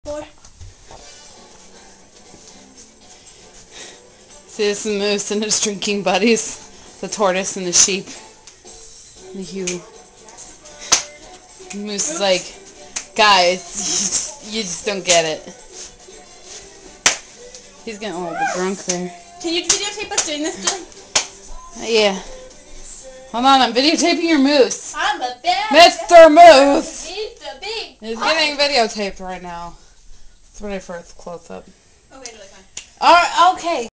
Moose and his Drinking Buddies
random video of my sister's stuffed animals